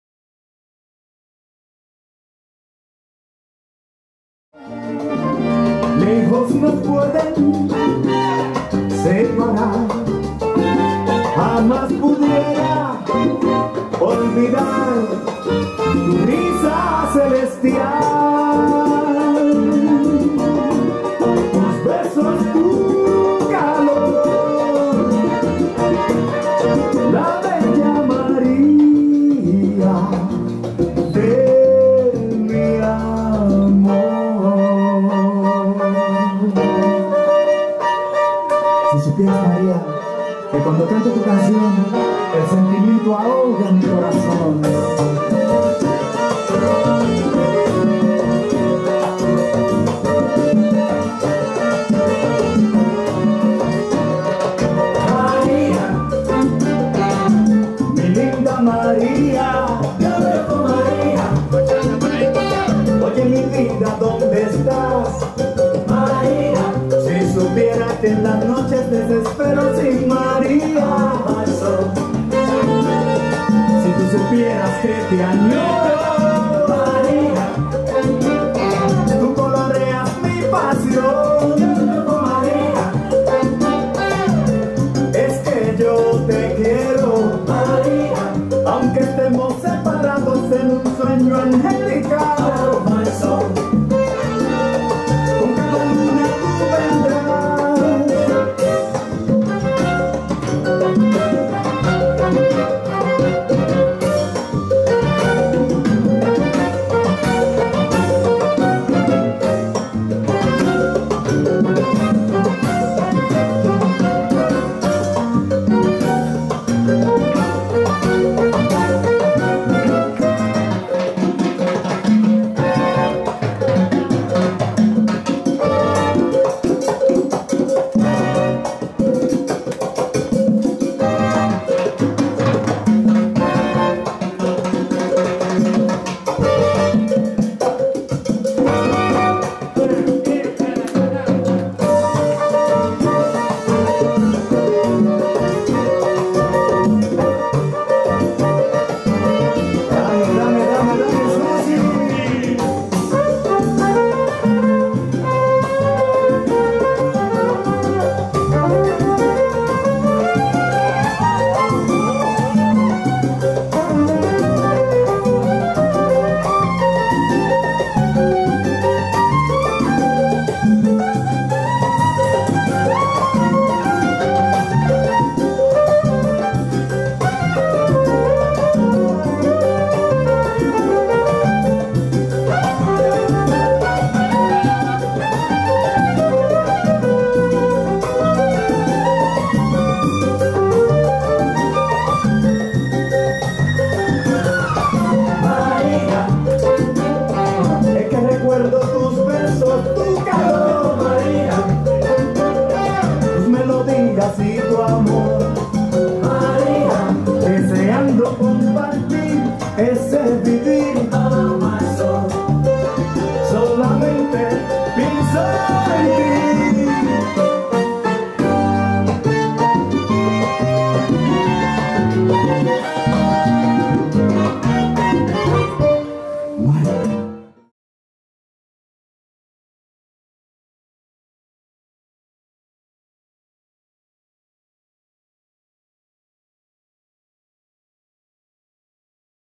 Sergey Bobkov and Kseniya Maltseva Salsa Dancing at 3 Front Relax 2019 in Hotel Don-Plaza, 04.08.19
Sergey Bobkov and Kseniya Maltseva Salsa Dancing at 3 Front Relax 2019, Party in Hotel Don-Plaza, Sunday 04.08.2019